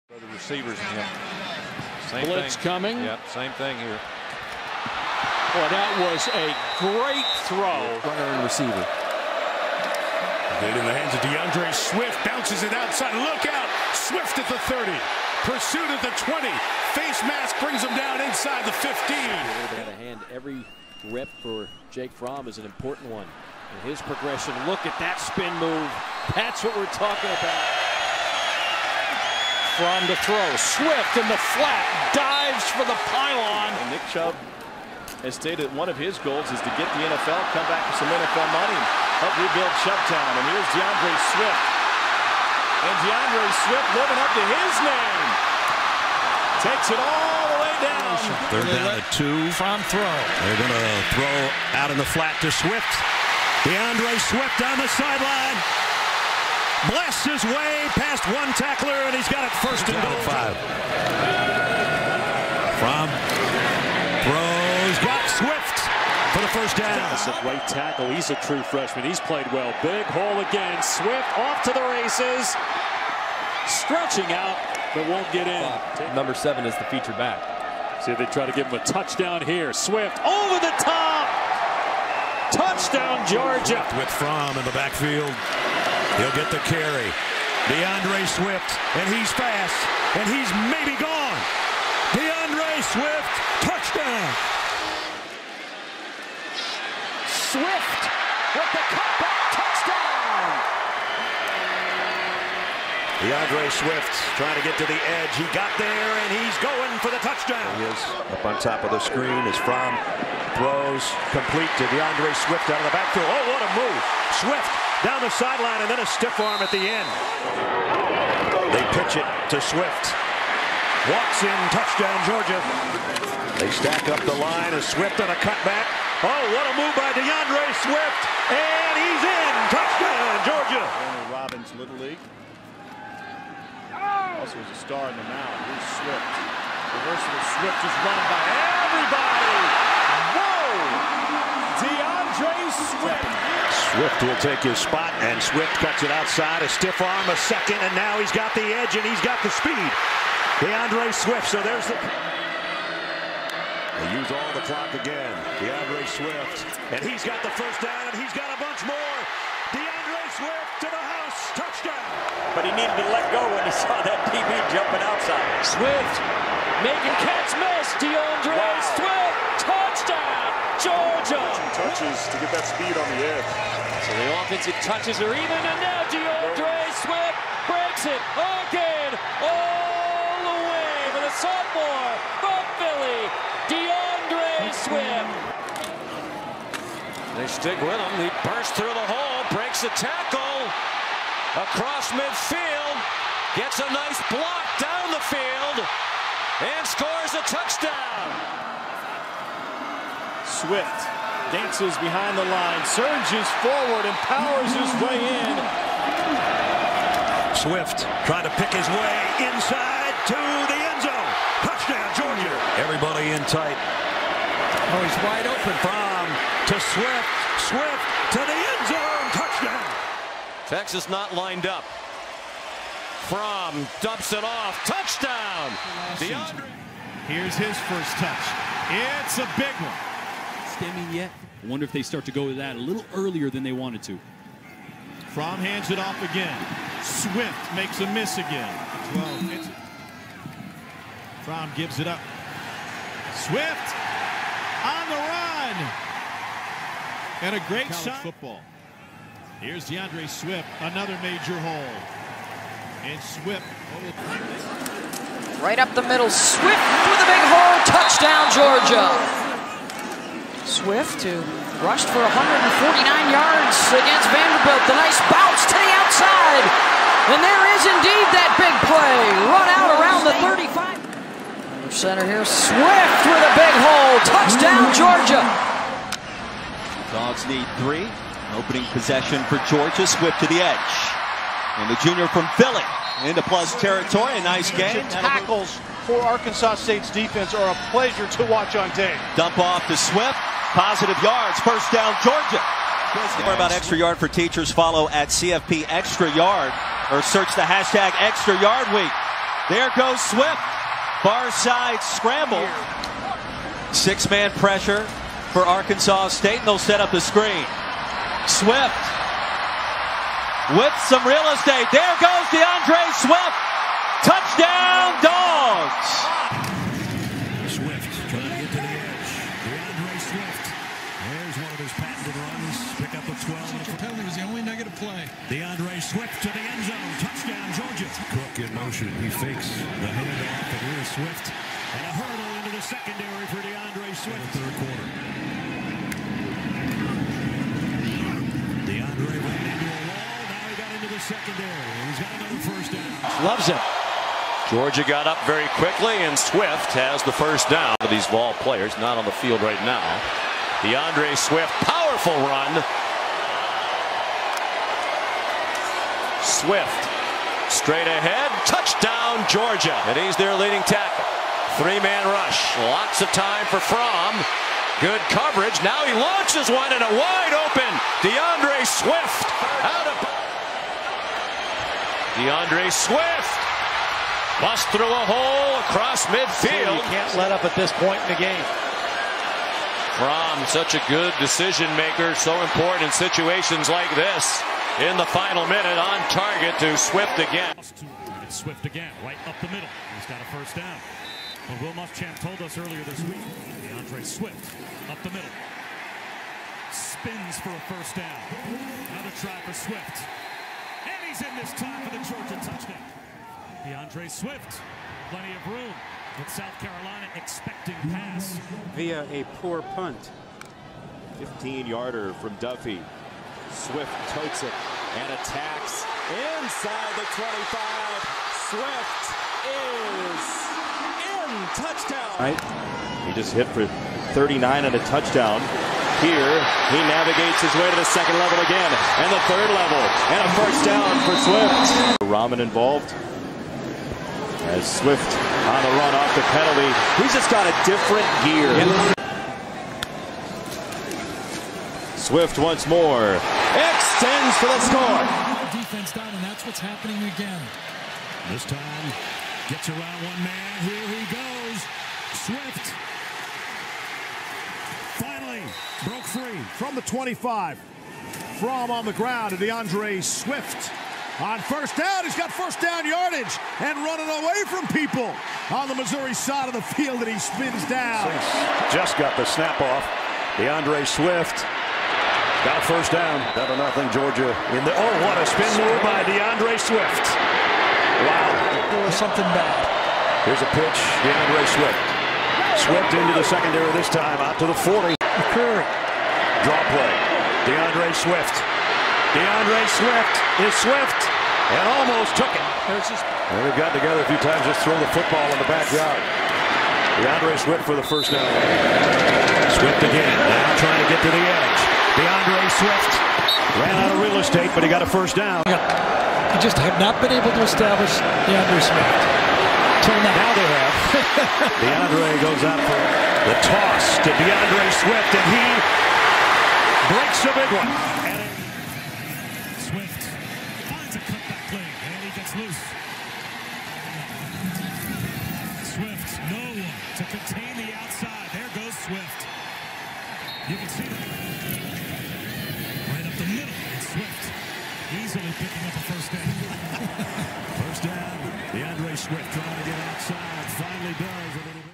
D'Andre Swift: Georgia Career Highlights from the Detroit Lions' 2nd round pick | SEC on CBS
Swift in name AND in style: D'Andre spent his time at the University of Georgia racking up yards and touchdowns. He earned first-team All-SEC honors for the Bulldogs in 2019, thanks to 1,218 yards and seven touchdowns on 196 carries (6.2 yards per attempt) and 24 catches for 216 yards (9.0 yards per catch) and a touchdown.